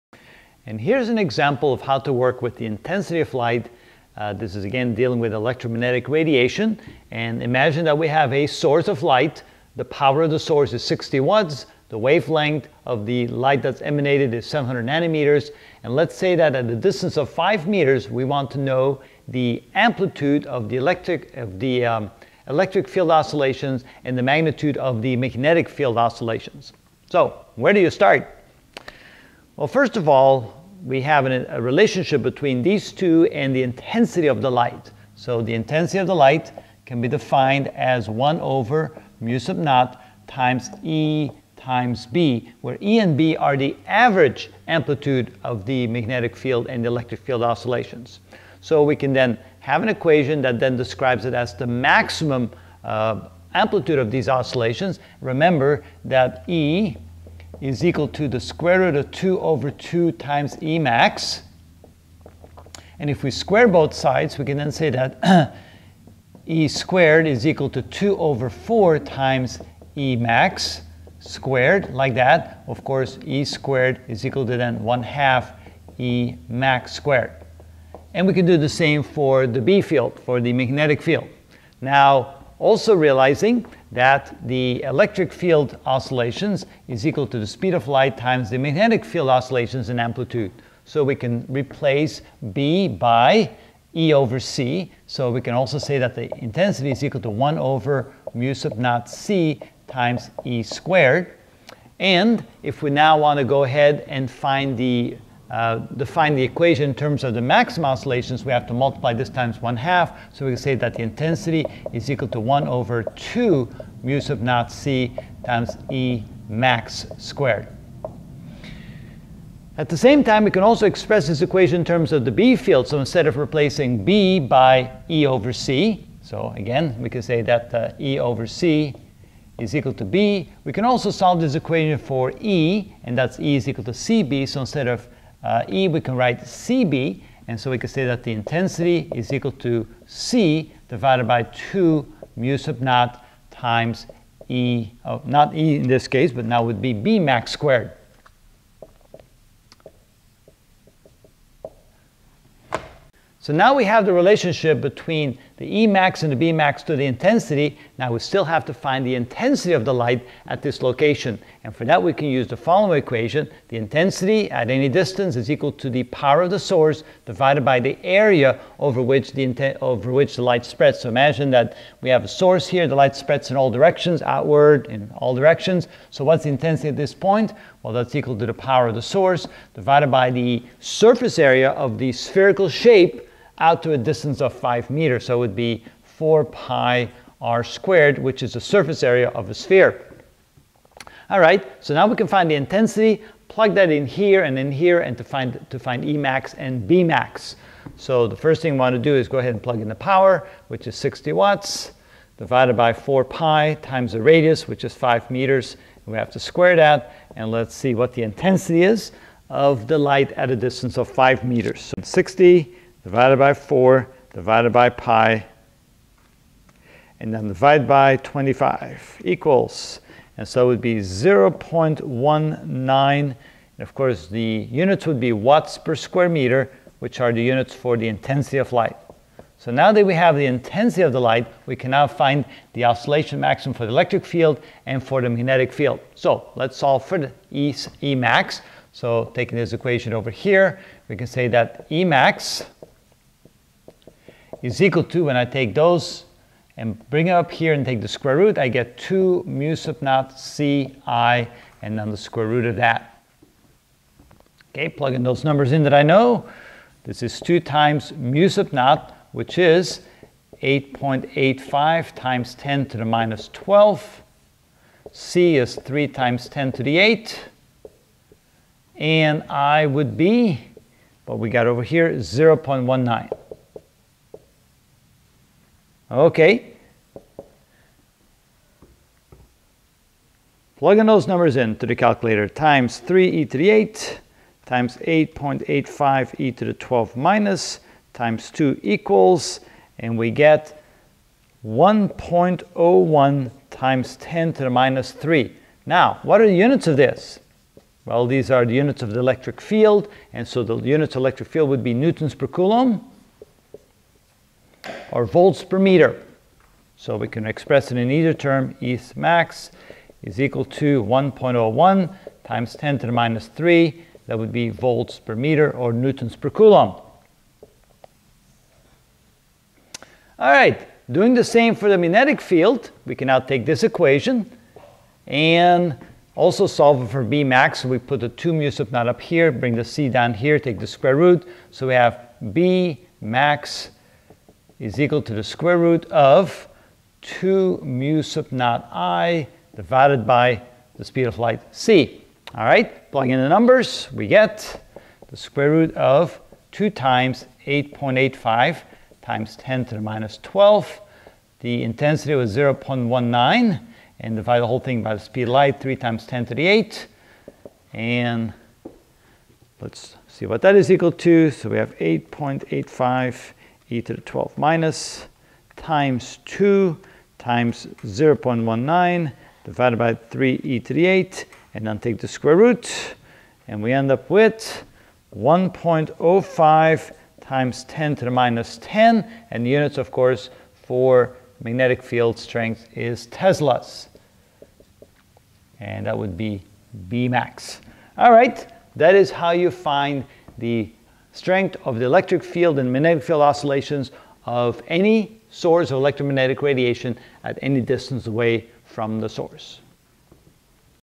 Physics 50 E&M Radiation (30 of 33) Intensity of Light Example
In this video I will calculate the Emax and Bmax 5m away from a light source of 60W and wavelength of 700nm.